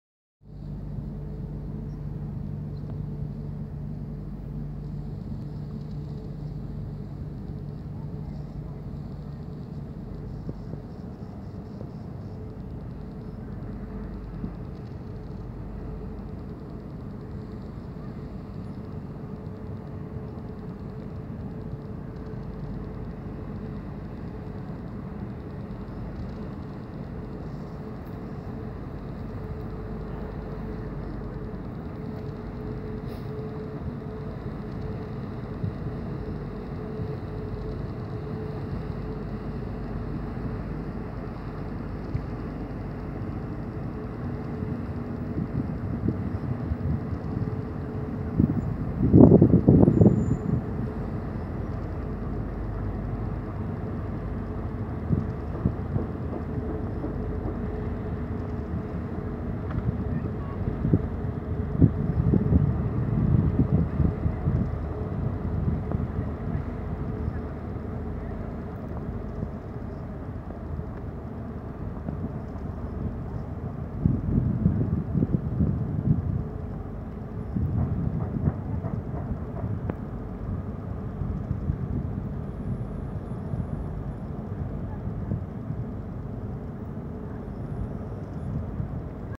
Statek "Mare Delfzijl" wchodzi do portu - Gdańsk Nowy Port - 27.09.2016
Statek "Mare Delfzijl" IMO 9214006 wchodzi do portu w Gdańsku. Obok niego przepływa holownik portowy "Bóbr" IMO 7645756.